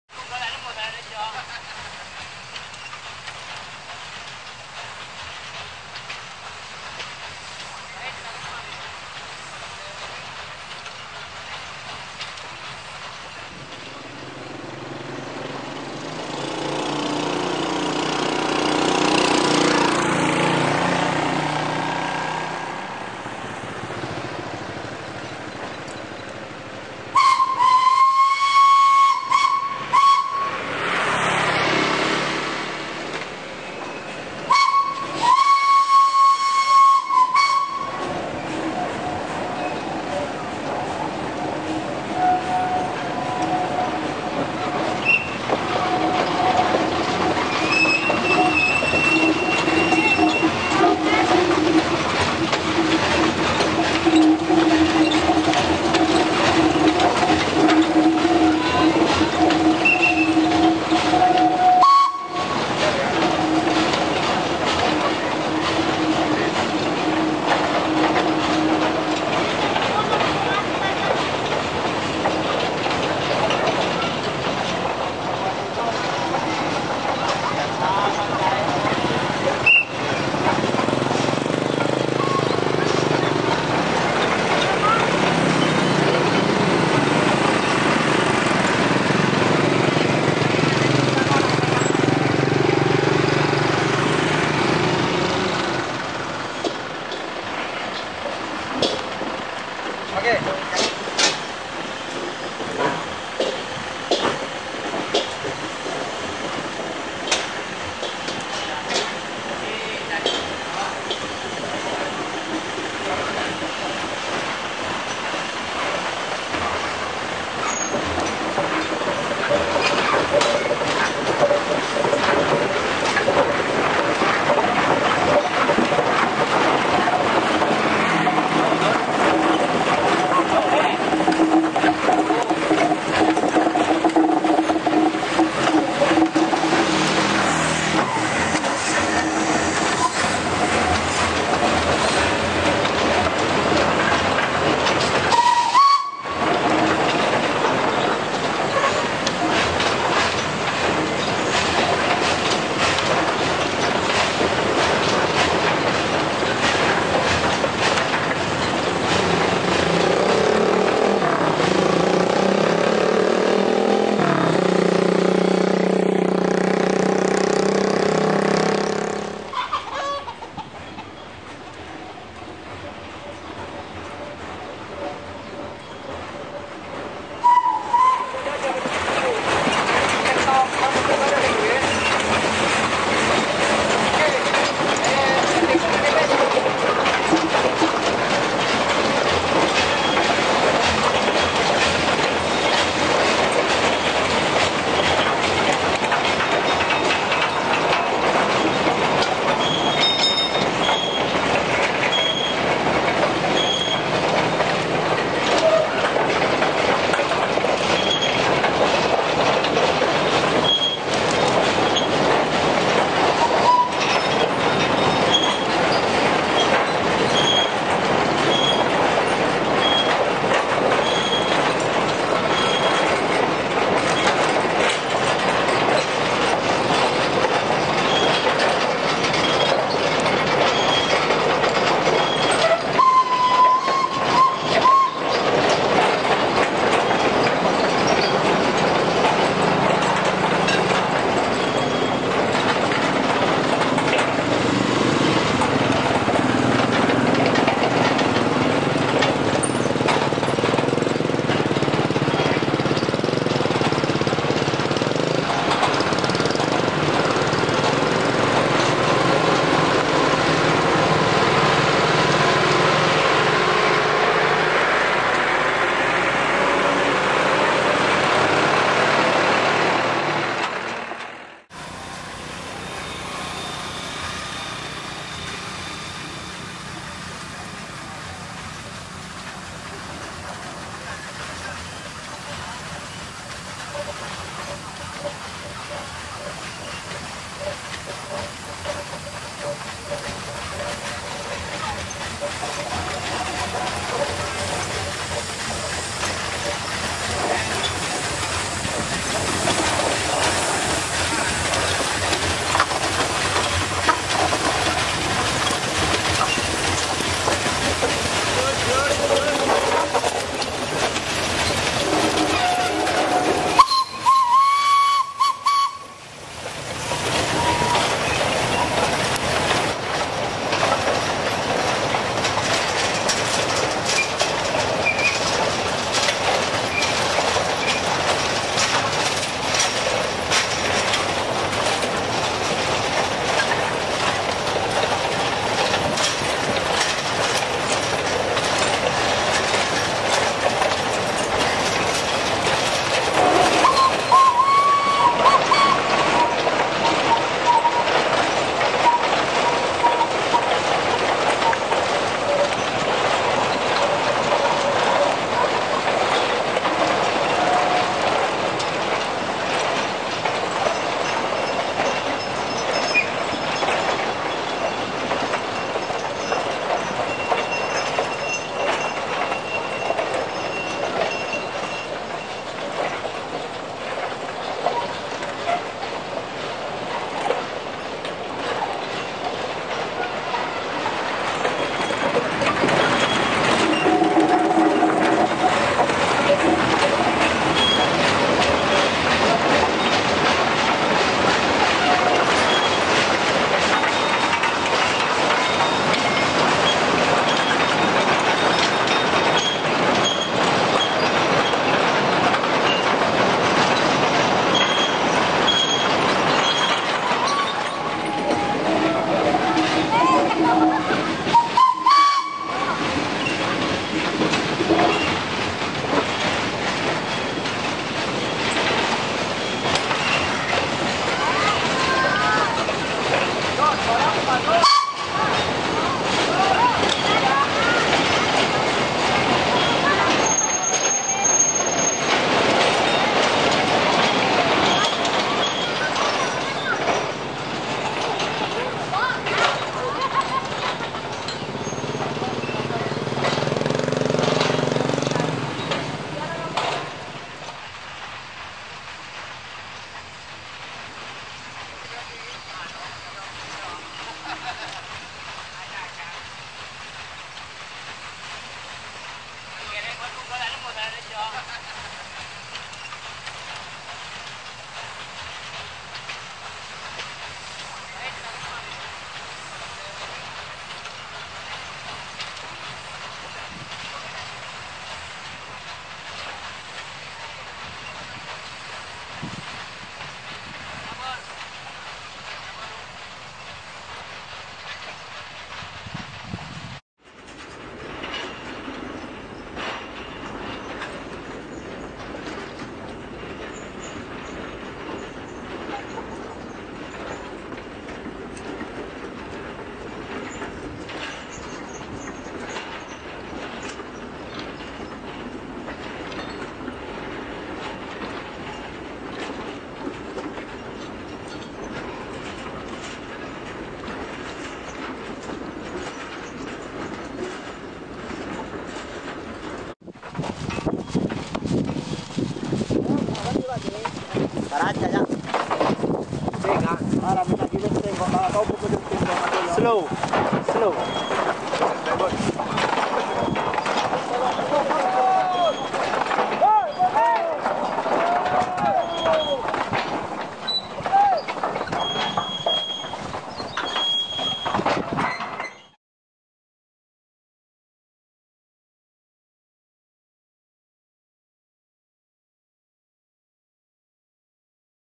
Olean - The Last Steam Operated Field Railway in Java - Pt.1
Olean, near Situbondo, was the last sugar mill in Java that used steam locomotives on lines out into the cane fields. By 2015, there was only one serviceable locomotive left, which could be hired to substitute for the job now normally performed by diesel engines. Olean No: 4 "SEMERU" O&K 0-8-0T (5440/1912), 700mm, is filmed here taking out empty wagons to the cane fields, where they are distributed by teams of Oxen / Water Buffalo on temporary tracks.
This task would normally be performed at sunrise, but obviously delayed for my benefit.

The mill closed completely after the 2016 harvest. It was last place that steam and oxen / water buffalo worked together.